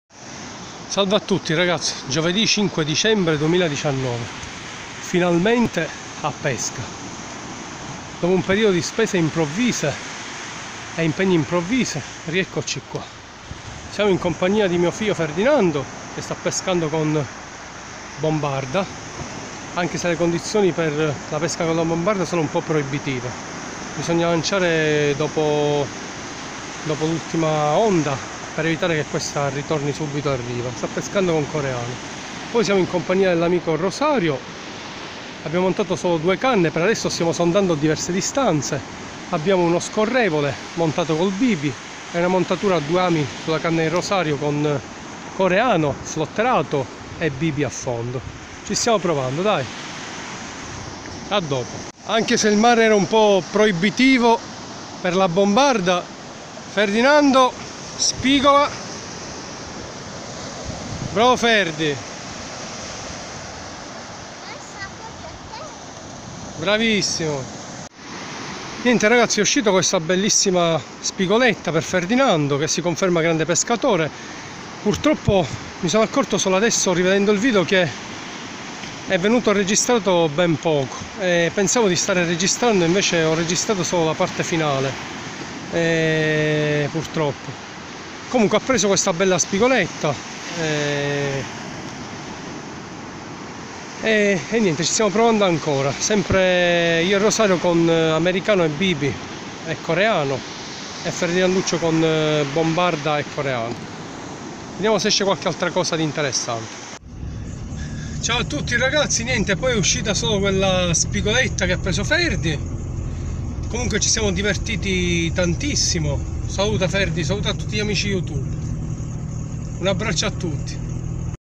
A volte l'allievo supera i "maestri". Spigola con bombarda da riva.
Finalmente a mare. Dopo un lungo periodo di astinenza, io e il mio amico Rosario riusciamo a organizzare una mezza giornata di pesca a surcasting. Con noi viene pure mio figlio Ferdinando, che ama la pesca come suo papà. Per tutta la giornata i cimini delle nostre canne rimangono fermi, nonostante io e Rosario alterniamo sugli ami coreano, bibi e americano, provando sia a fondo sia  flotterando le esche. Unico lampo della giornata è una bella spigoletta arrivata sulla cannetta di Ferdinando che pescava con bombarda semiaffondante, finale dello 0.17 e un amo della Gamakatsu dell' 8. Le condizioni per il tipo di pesca del bambino erano un po' proibitive, visto il mare abbastanza mosso, ma lanciando dopo l'ultimo frangente si riusciva a rallentare un po' il rapido spiaggiamento della bombarda da parte dell'azione delle onde. Alla fine viene premiata la caparbietà del piccolo di volere a tutti i costi provare quel tipo di pesca con quel mare e la sua tecnica che, seppur  ancora immatura, ha dato comunque i suoi frutti. Alla prossima, speriamo presto!